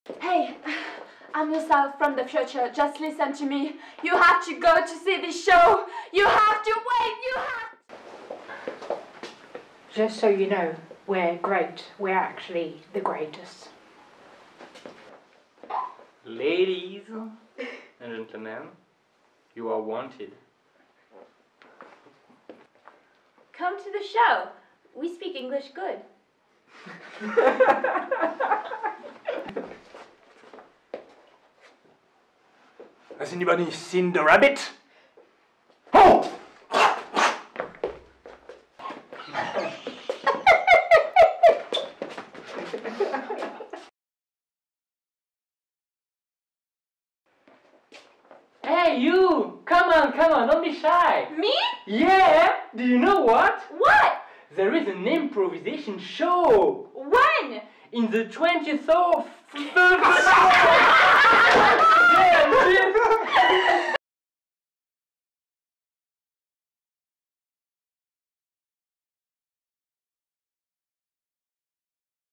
Teaser - IMPROVisation in English, May-24th 2019
Grenoble IMPROV Show is our English speaking show running in Grenoble. The shows is physical and action-packed: perfect for non-native English speakers too! Our shortform show delivers a fast-paced, highly interactive experience with rich narrative and character development. The show features all your favourites categories.